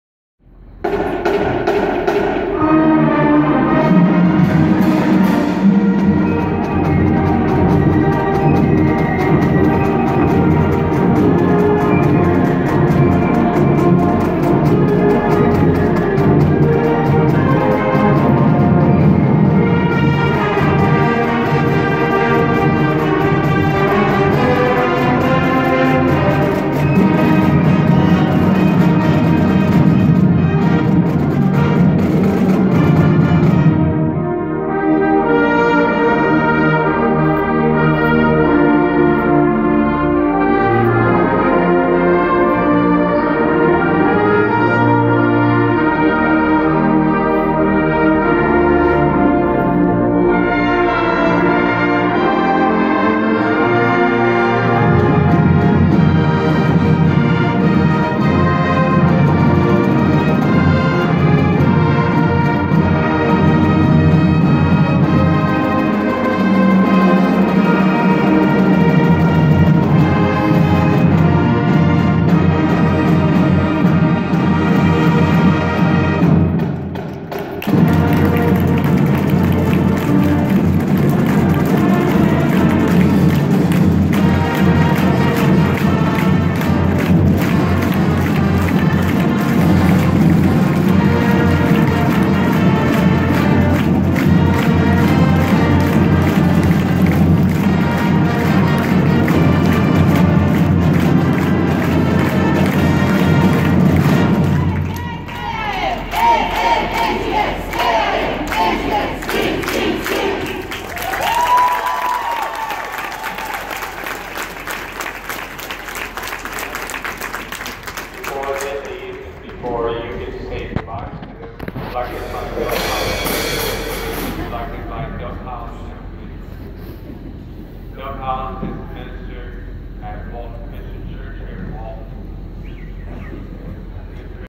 Lewis Cass Marching Kings Macy's Show Preview
Lewis Cass Marching Kings are headed to New York to march in the 89th annual Macy's Thanksgiving Day Parade. Before they left they performed a preview show for friends and family. (Video by Phill Dials for Cass County Communication Network)